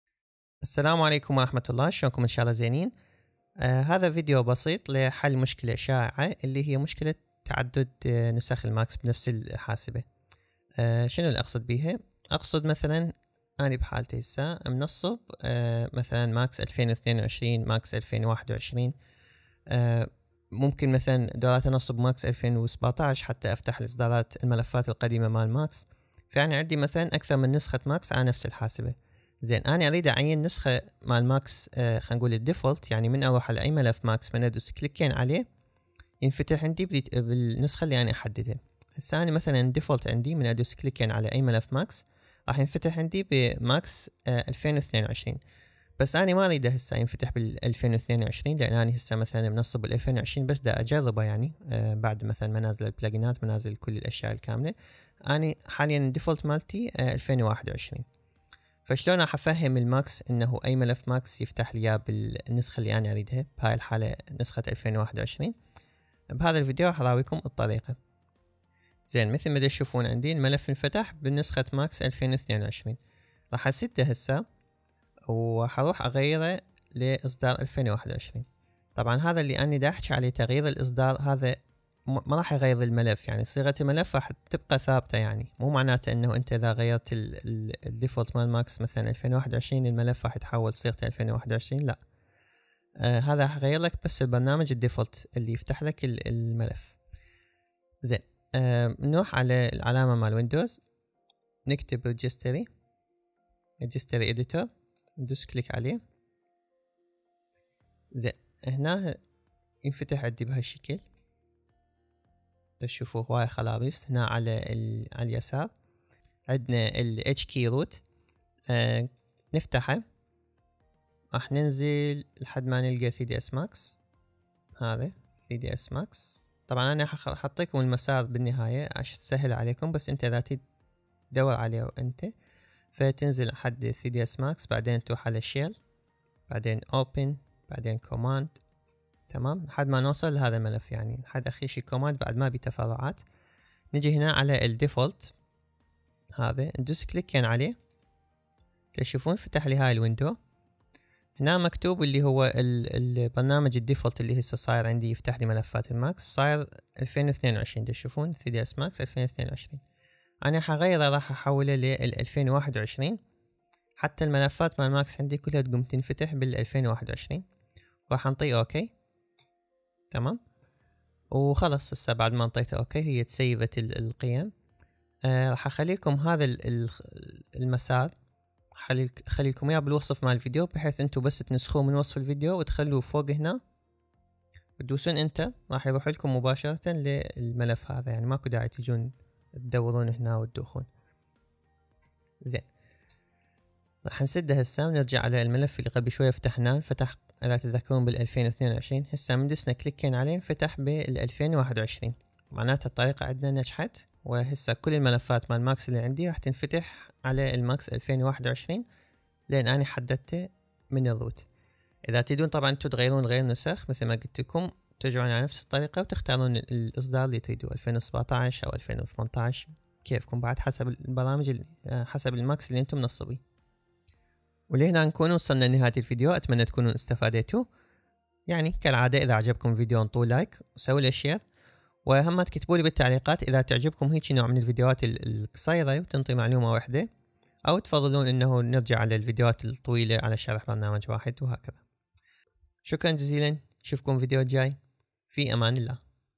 طريقة تحديد نسخة الماكس الاساسية لفتح الملفات
بهذا الفديو راح اشرحلكم طريقة تحديد النسخة الاساسية للماكس الي من خلالها تفتح ملفات الماكس

المسار المشروح في الفديو
Computer\HKEY_CLASSES_ROOT\3dsmax\Shell\Open\Command

مواضيع اخرى:
📌 تحميل البرنامج من قناة التلكرام

مواضيع اخرى:
افضل تجميعات الدسكتوب 📌
افضل الكتب المعمارية 📌
مواصفات جهازي 🔥